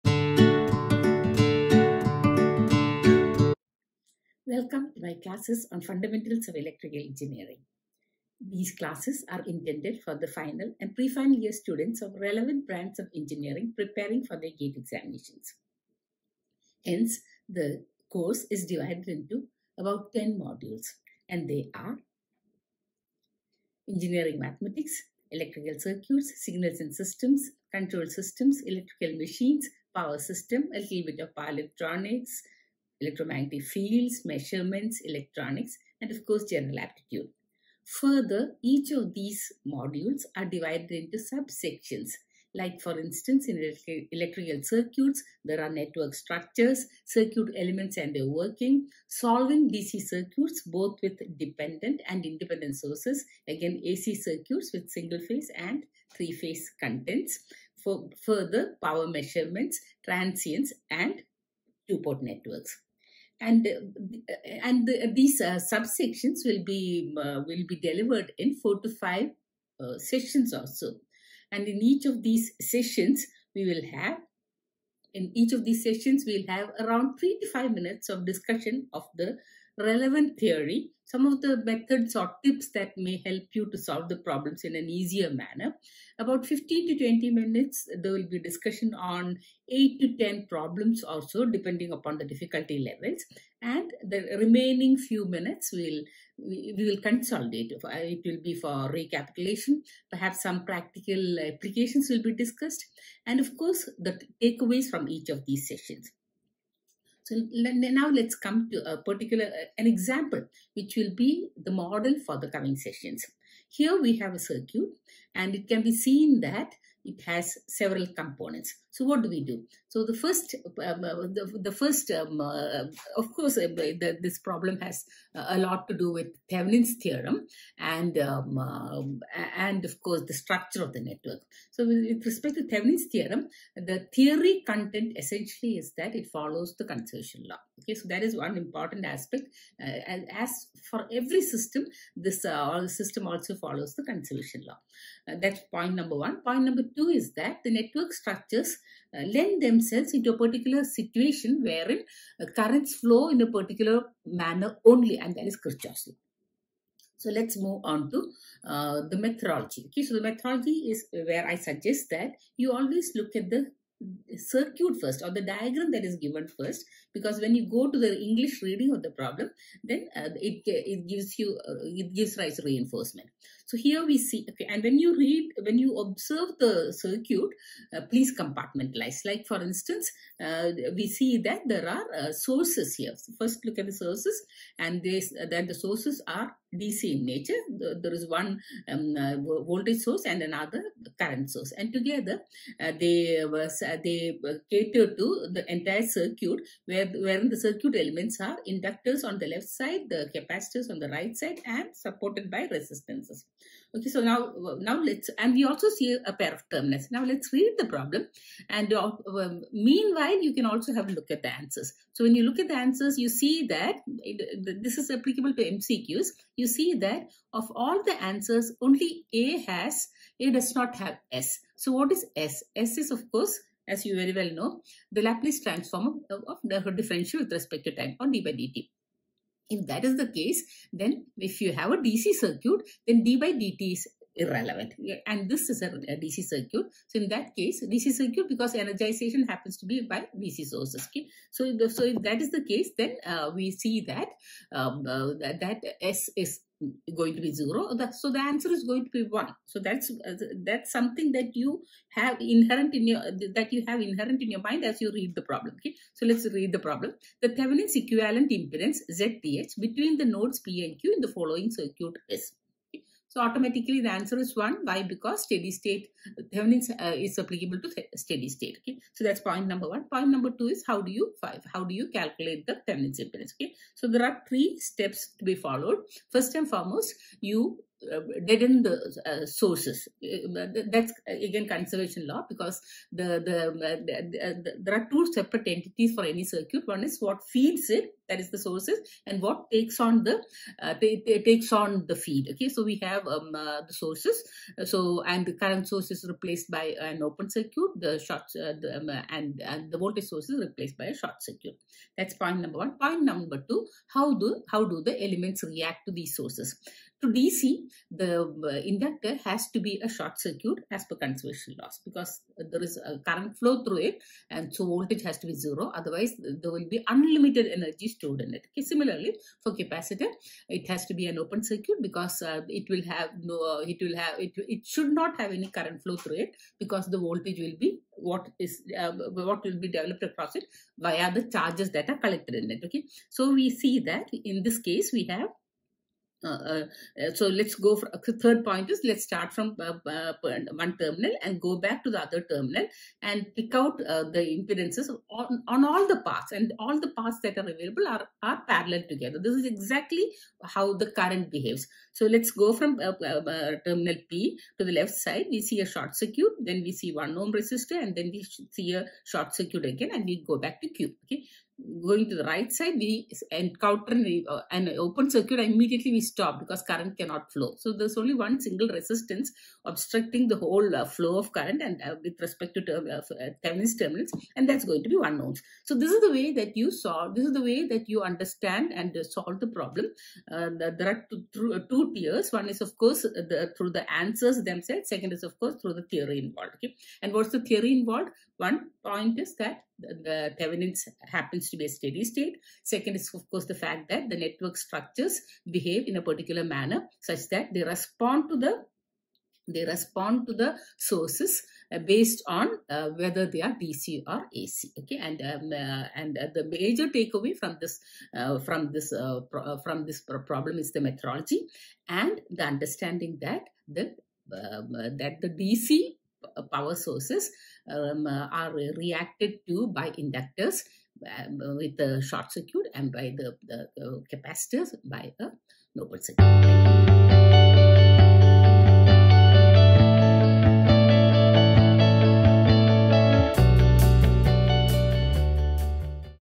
1 GATEIntroduction to a course in fundamentals of EE SD 480p
This is a series of videos. The contents are designed for preparing for GATE exams. The modules are
1. Networks & Circuits
2. Signals & Systems
3. Control Systems
4. Engineering. Maths
5. Aptitude: Verbal, Numerical
6. Electrical Machines
7. Power Systems
8. Power Electronics, Measurements
9. Electronics, electro-magnetics, etc.